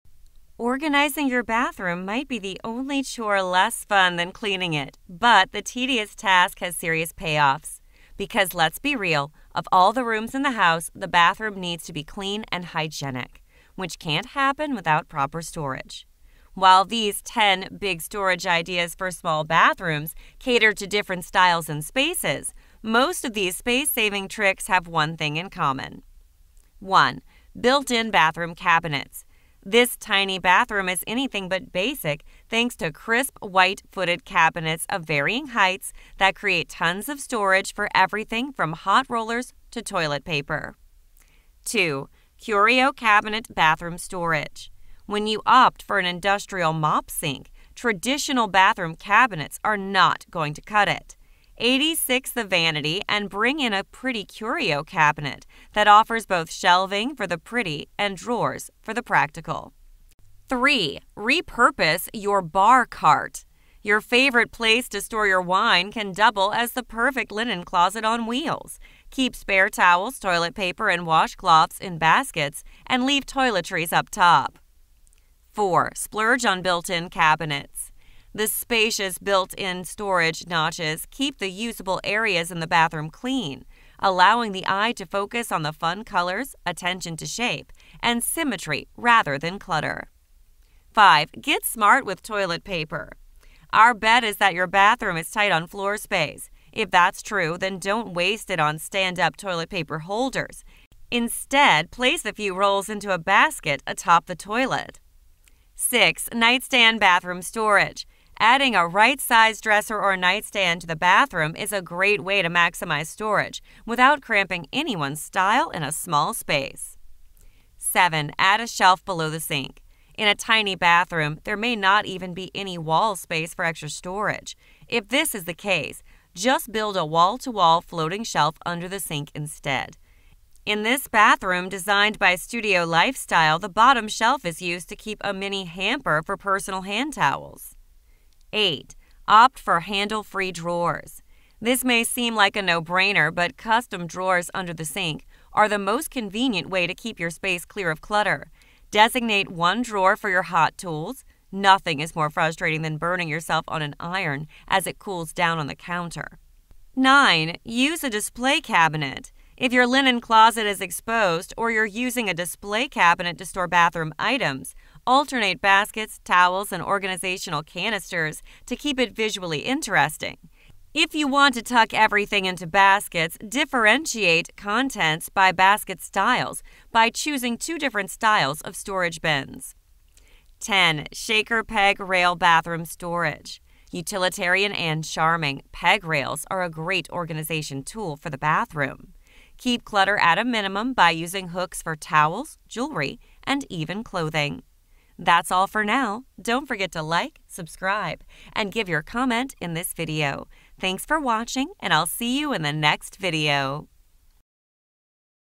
10 Small Storage Ideas Bathrooms @homeideas3936 @lavateraatelier1621
Small Storage Ideas for Small Bathrooms. Organizing your bathroom might be the only chore less fun than cleaning it but the tedious task has serious payoff. Because let's be real: Of all the rooms in the house, the bathroom needs to be clean and hygienic, which can't happen without proper storage. While these 10 Big Storage Ideas for Small Bathrooms cater to different styles and spaces, most of these space-saving tricks have one thing in common.